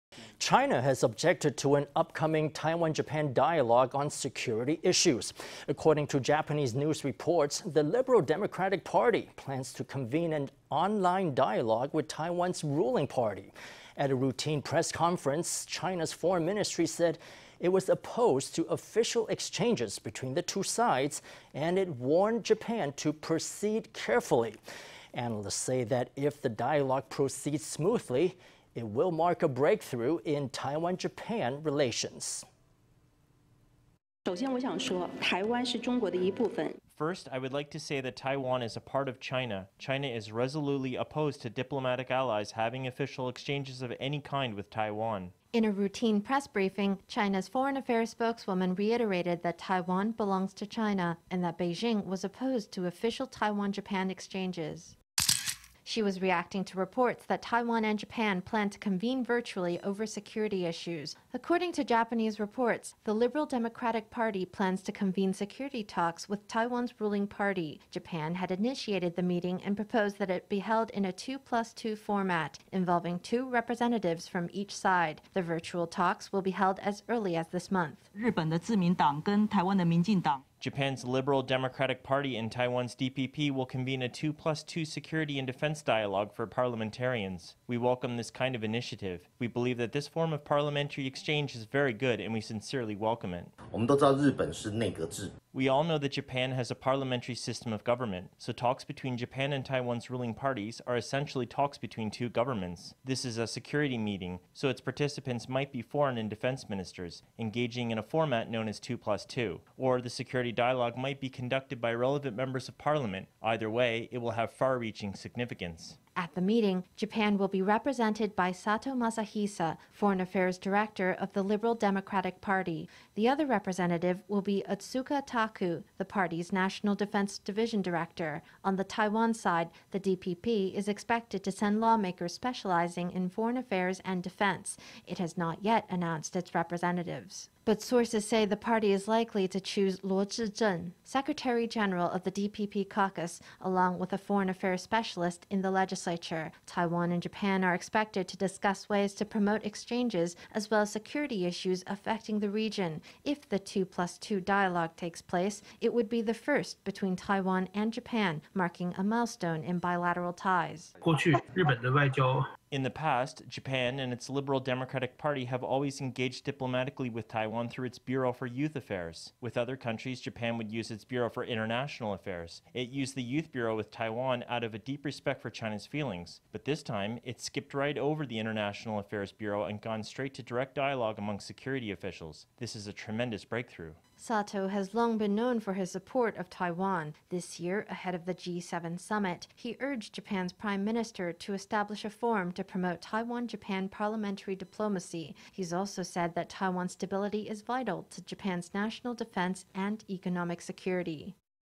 China warns over Taiwan-Japan security dialogue
China has objected to an upcoming Taiwan-Japan dialogue on security issues. According to Japanese news reports, the Liberal Democratic Party plans to convene an online dialogue with Taiwan''s ruling party. At a routine press conference, China''s foreign ministry said it was opposed to official exchanges between the two sides, and it warned Japan to proceed carefully. Analysts say that if the dialogue proceeds smoothly, it will mark a breakthrough in Taiwan-Japan relations.

Hua Chun-ying
China Ministry of Foreign Affairs spokeswoman
First I would like to say that Taiwan is a part of China. China is resolutely opposed to diplomatic allies having official exchanges of any kind with Taiwan.

In a routine press briefing, China’s foreign affairs spokeswoman reiterated that Taiwan belongs to China, and that Beijing was opposed to official Taiwan-Japan exchanges.

She was reacting to reports that Taiwan and Japan plan to convene virtually over security issues. According to Japanese reports, the Liberal Democratic Party plans to convene security talks with Taiwan’s ruling party. Japan had initiated the meeting and proposed that it be held in a “two-plus-two” format, involving two representatives from each side. The virtual talks will be held as early as this month.

Liu Shyh-fang
DPP lawmaker
Japan’s Liberal Democratic Party and Taiwan’s DPP will convene a “two-plus-two” security and defense dialogue for parliamentarians. We welcome this kind of initiative. We believe that this form of parliamentary exchange is very good and we sincerely welcome it.

Wang Ting-yu
DPP lawmaker
We all know that Japan has a parliamentary system of government. So talks between Japan and Taiwan’s ruling parties are essentially talks between two governments. This is a security meeting, so its participants might be foreign and defense ministers, engaging in a format known as “two-plus-two.” Or the security dialogue might be conducted by relevant members of parliament. Either way, it will have far-reaching significance.

At the meeting, Japan will be represented by Sato Masahisa, foreign affairs director of the Liberal Democratic Party. The other representative will be Otsuka Taku, the party’s national defense division director. On the Taiwan side, the DPP is expected to send lawmakers specializing in foreign affairs and defense. It has not yet announced its representatives.

But sources say the party is likely to choose Lo Chih-cheng, secretary-general of the DPP caucus, along with a foreign affairs specialist in the legislature. Taiwan and Japan are expected to discuss ways to promote exchanges, as well as security issues affecting the region. If the “two-plus-two” dialogue takes place, it would be the first between Taiwan and Japan, marking a milestone in bilateral ties.

Yaita Akio
Sankei Shimbun Taipei bureau
In the past, Japan and its Liberal Democratic Party always engaged diplomatically with Taiwan through its bureau for youth affairs. With other countries, Japan would use its bureau for international affairs. It used the youth bureau with Taiwan out of a deep respect for China’s feelings. But this time, it’s skipped right over the international affairs bureau and gone straight to direct dialogue among security officials. This is a tremendous breakthrough.

Sato has long been known for his support of Taiwan. This year, ahead of the G7 summit, he urged Japan’s prime minister to establish a forum to promote Taiwan-Japan parliamentary diplomacy. He’s also said that Taiwan’s stability is vital to Japan’s national defense and economic security.